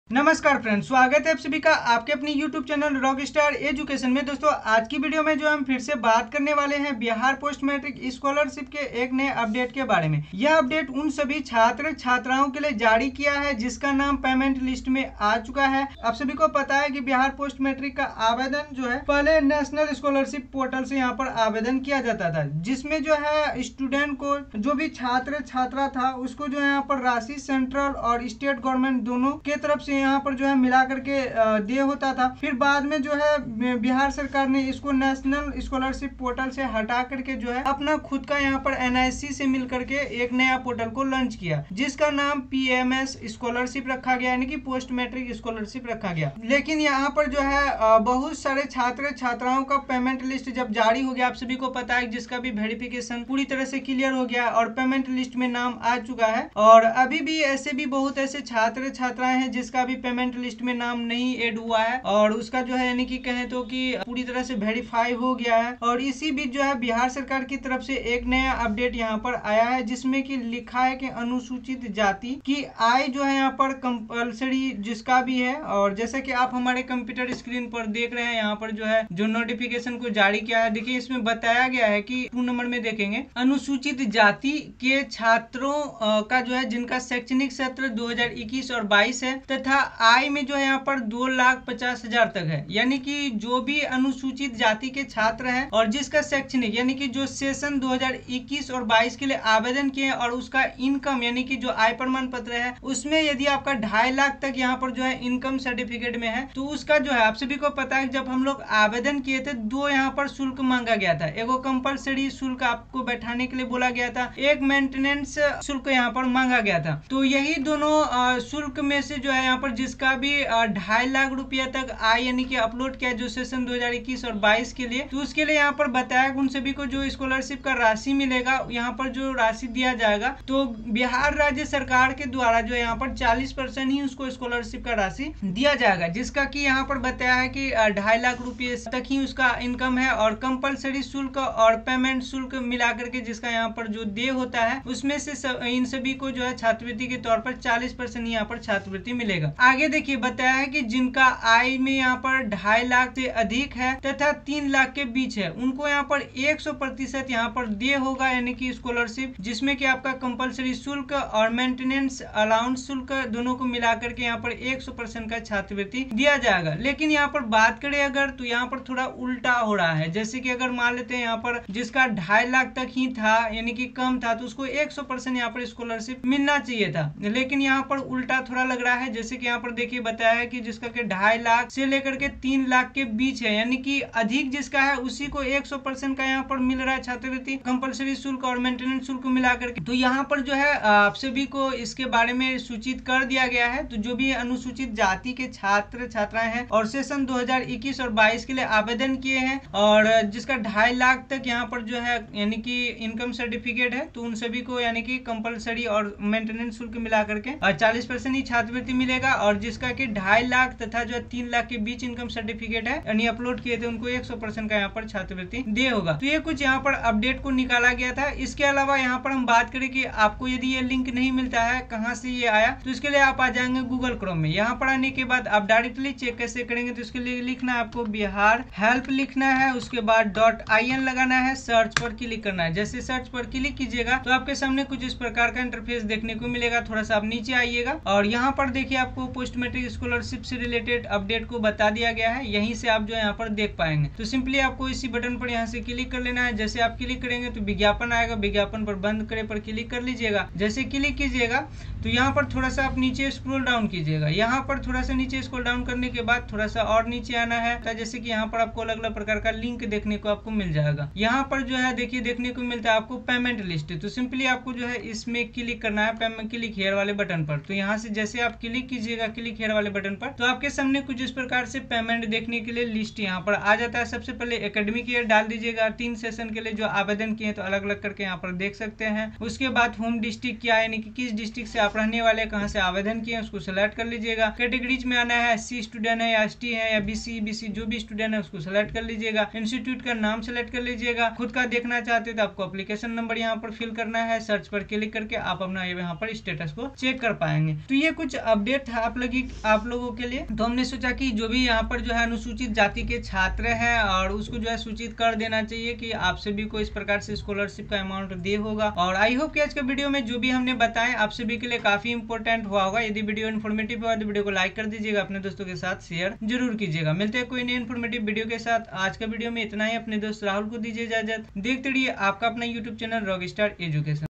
Bihar Post matric scholarship 3 लाख income वाले को 100% स्कॉलरशिप 2.50 लाख वाले को 40% ही😭देख लो
Likes 👍
Comments 📝
.Jaroor kare 🤗🤗🤗😘.
Hamare Saath Social Media se Jurne ke liye Niche link per click kare 👇👇